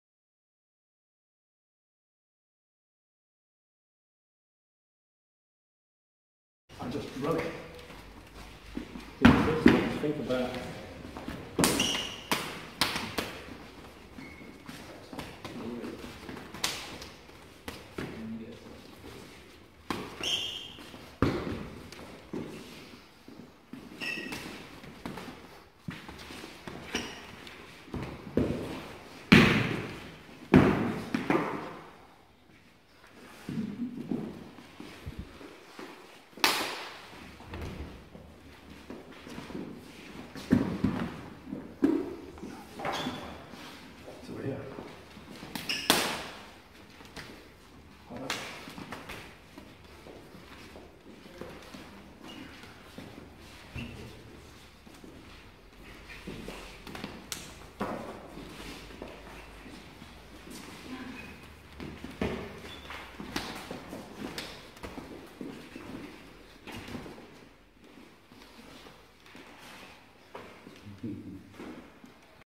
Letting the hands become natural - Gary Lam Wing Chun Nottingham, UK
Sifu Dean Pugh demonstrates hands naturally changing, controlling and striking.